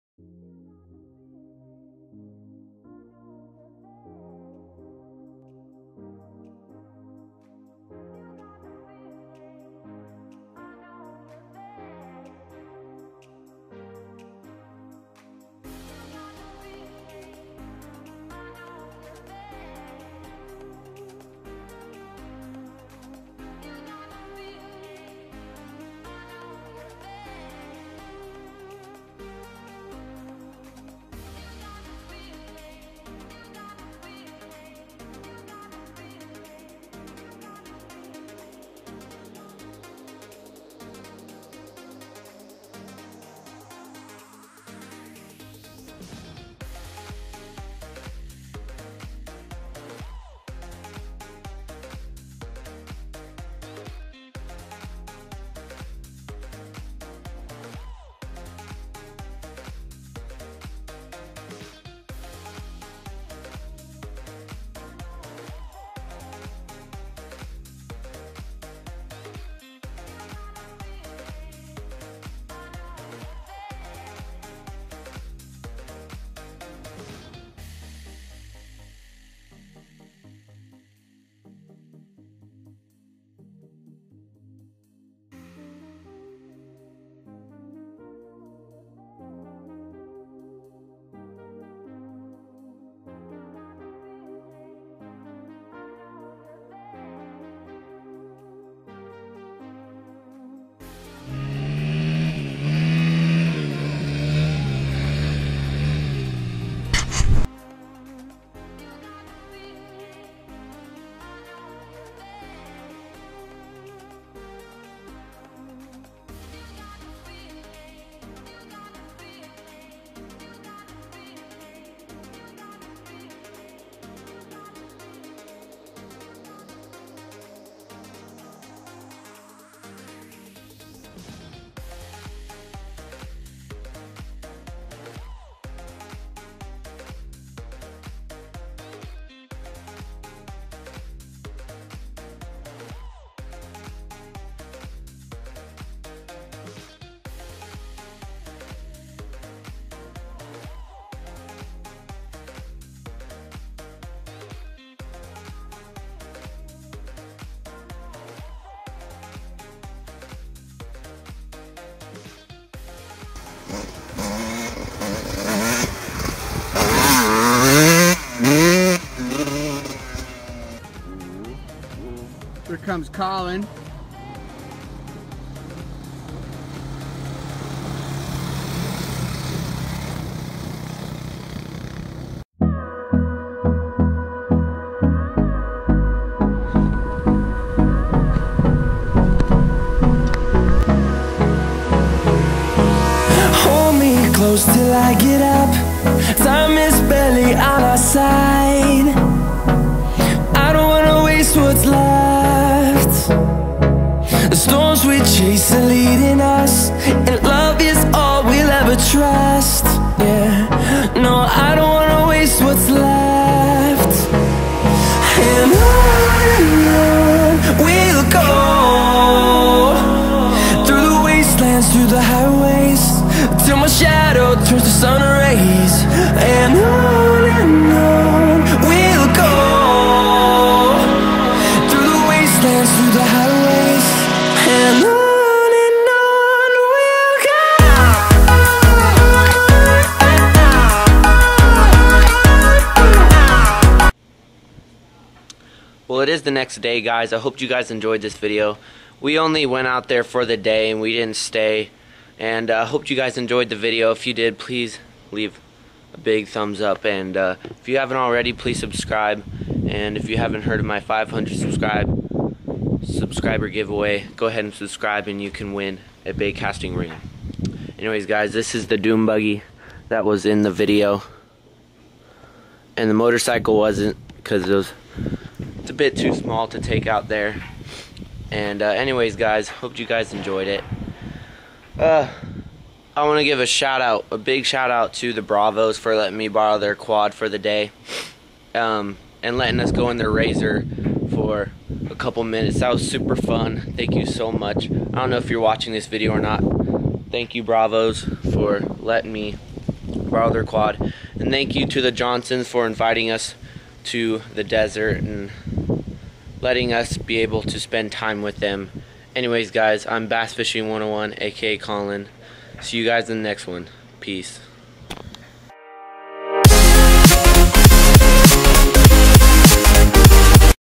Ocotillo Wells Desert Trip! - Dirt Bikes, Quads, Buggy's, RZR's, & more! [HD]
This trip was awesome! Glad I got some of it on film and was able to make an awesome video! Hope you liked it!

Bass Fishing 101 out...